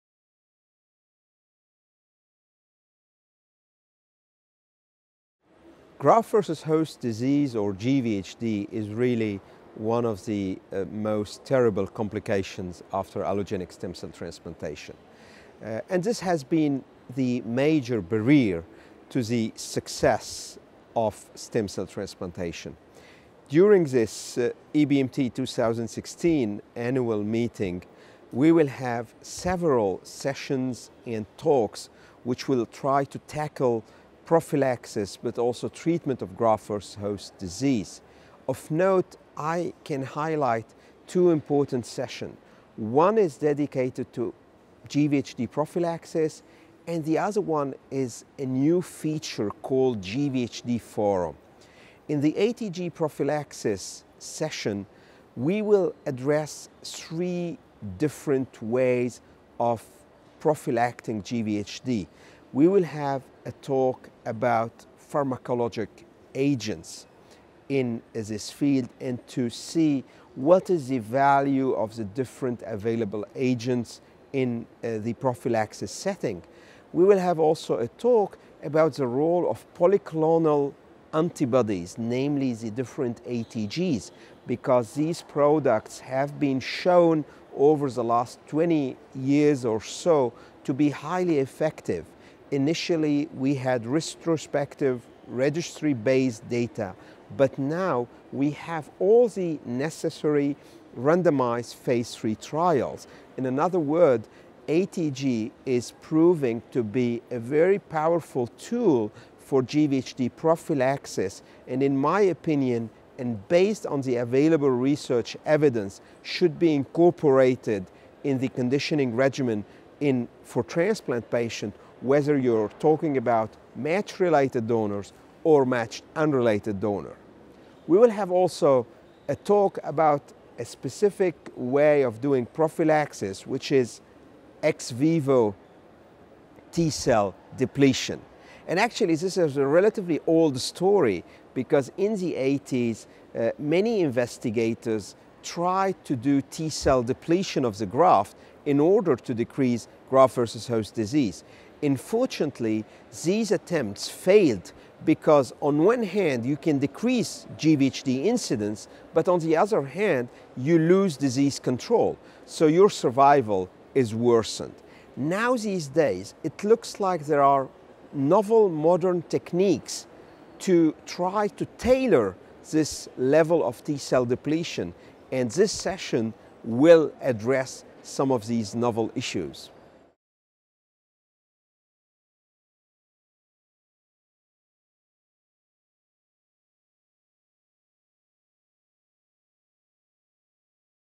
Graft-versus-host disease (GvHD): Advances at EBMT 2016
At the 42nd Annual Meeting of the European Society for Blood and Marrow Transplantation (EBMT), held in Valencia, Spain, EBMT 2016 President Mohamad Mohty, MD, PhD, from Saint-Antoine Hospital, Paris, France, discusses graft-versus-host disease (GvHD), a complication that can occur after allogeneic transplantation and be a major barrier to the success of stem cell transplantation. Dr Mohty gives an overview of EBMT 2016 discussions, which address GvHD prophylaxis, pharmacologic agents and the role of polyclonal antibodies.